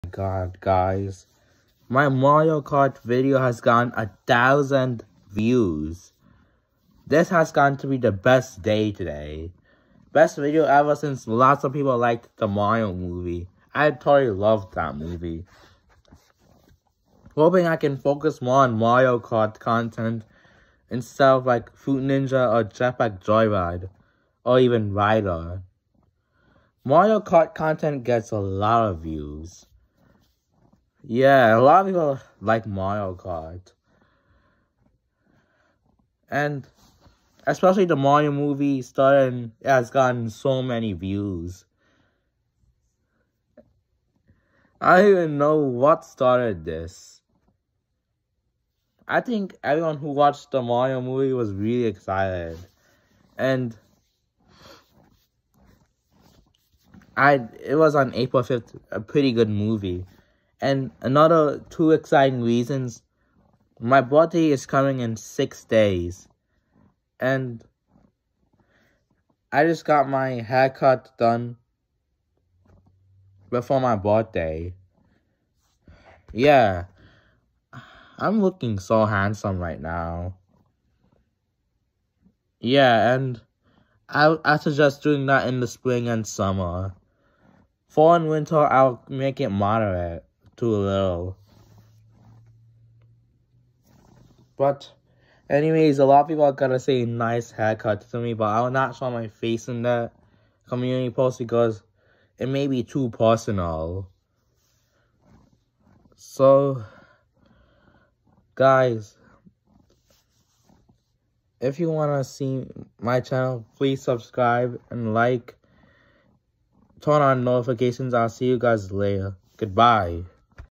2 Exciting Reasons (read in description)
1. My Mariokart Video has Gotten 1K Views
2. My Birthday is coming in 6 days, and I got a haircut